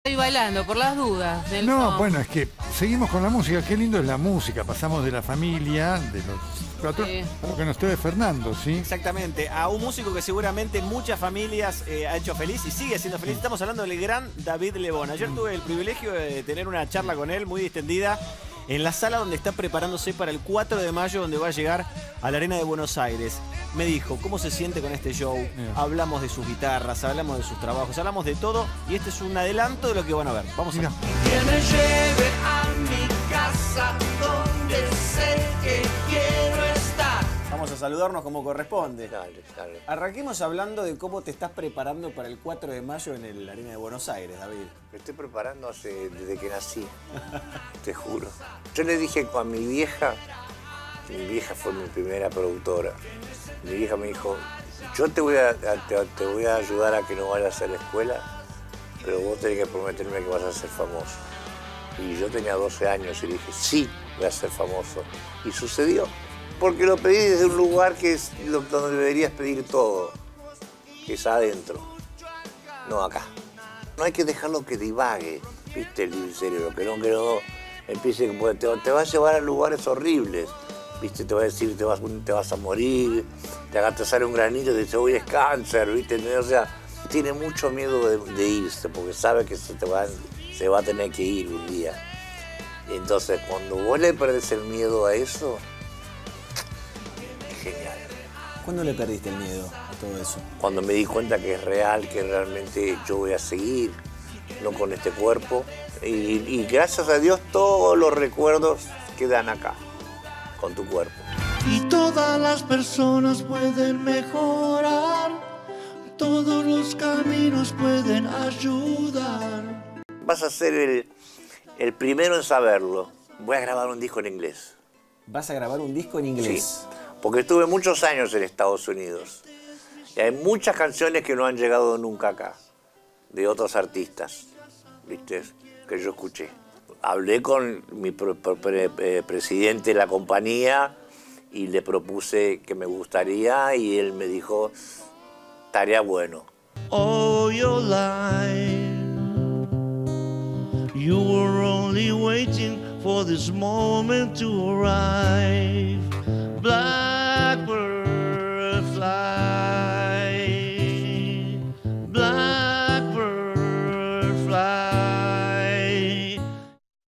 DAVID LEBÓN SE PRESENTARÁ EN EL ARENA: "Le prometí a mi vieja que iba a ser famoso"
Informe emitido el jueves 4 de abril de 2024 en Telenoche.

Fer Molinero tuvo el privilegio de entrevistar a David Lebón y adelantó cómo se prepara para el show que brindará en el Movistar Arena, el 4 de mayo de 2024.